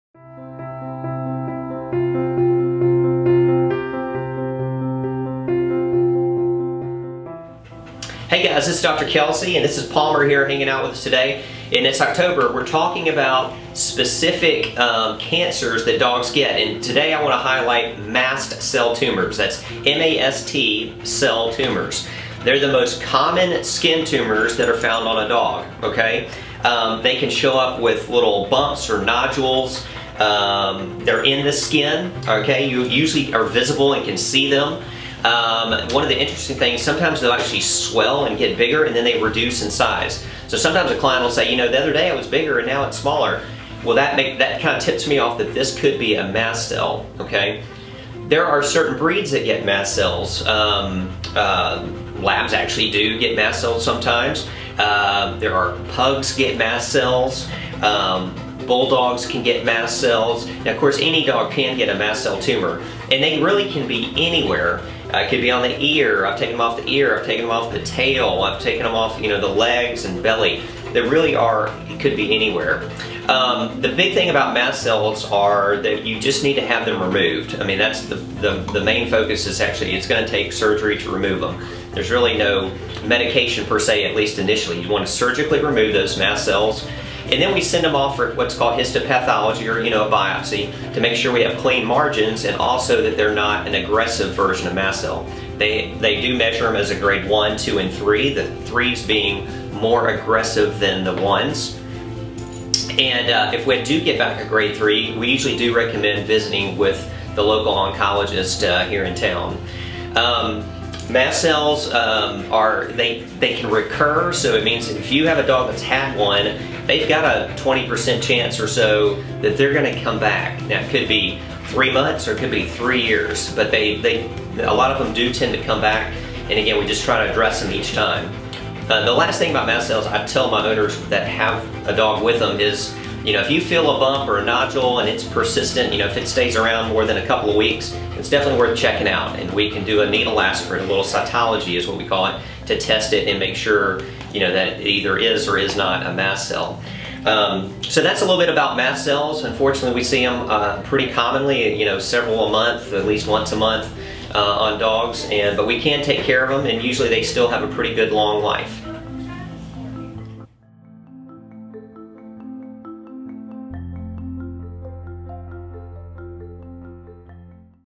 Canine Cancer: Mast Cell Tumor
Dr. Kelsey explains a bit about mast cell tumors, one of the most common types of cancer in dogs.